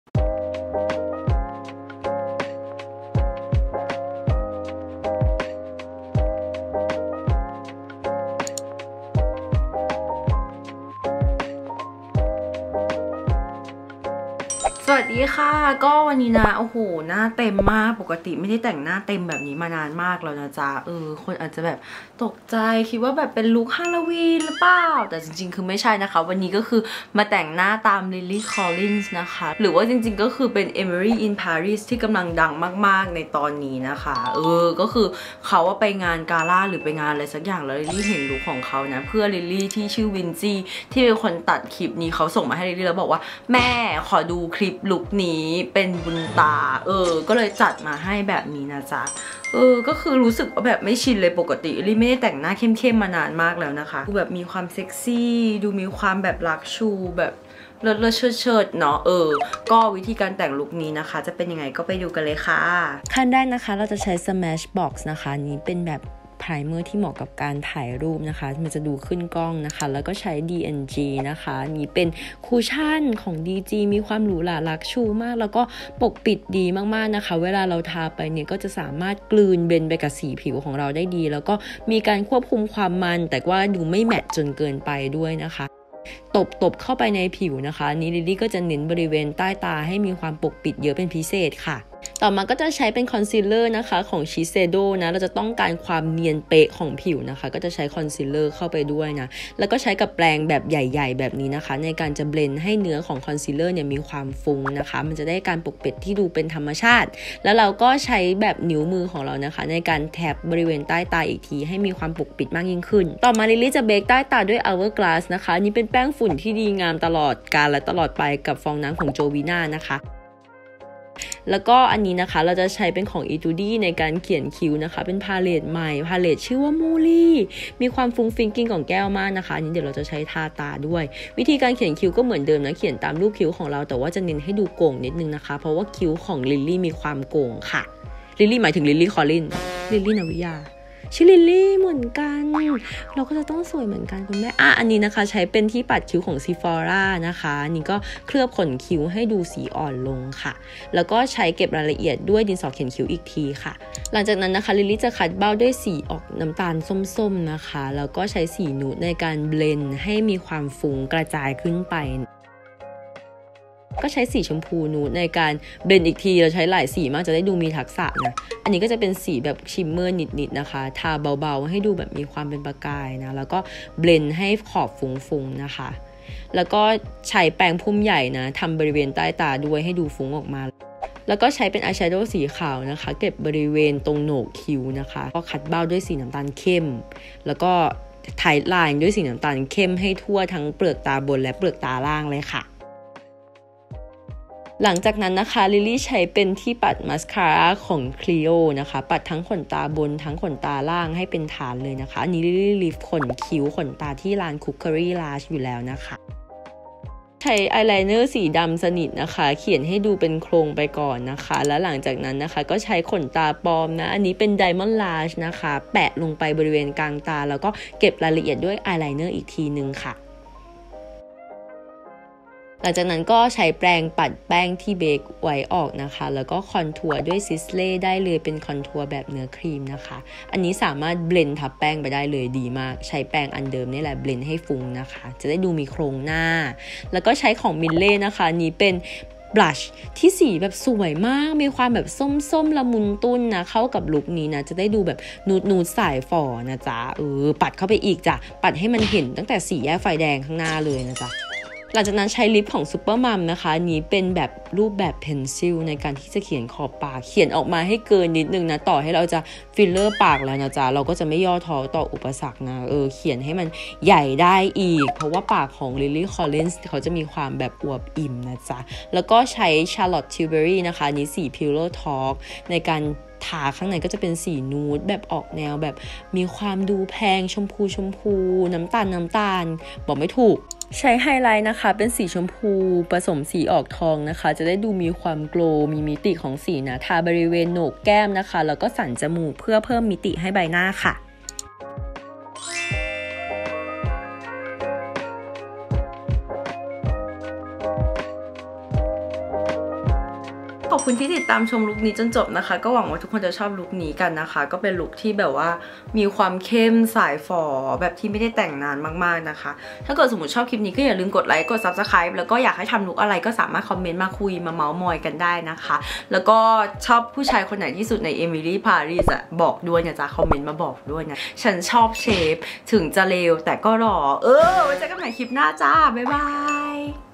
How to:แต่งหน้าสายฝอ2020 Emily in paris (Lily collins)| Lily nawiya
Lily collins look

Smash box
Photo Finish Foundation Primer Radiance
ขนาด: 30ml/1oz 1739 THB

DOLCE & GABBANA
คุชชั่น Preciouskin Perfect Finish Cushion Foundation 2610 THB

SHISEIDO
Synchro Skin Self-Refreshing Dual-Tip Concealer
1,300 THB

HOURGLASS
Veil Translucent Setting Powder
1,900 THB

Jovina extra soft sponge 350 THB

Etude house Play Color Eyes Muhly Romance
890 THB

CLIO : Kill Lash Superproof Mascara
299 THB

Creer Beaute Sailor Moon Miracle Romance Spiral Heart Moon Rod Liquid Eyeliner
385 THB

ขนตาปลอม Diamond lashes 590 THB

Supermom velvet crayon  RC10
129 THB

Charlotte tilbury lipstick pillow talk 1,650 THB

Burberry Essential Glow Palette 3,000 THB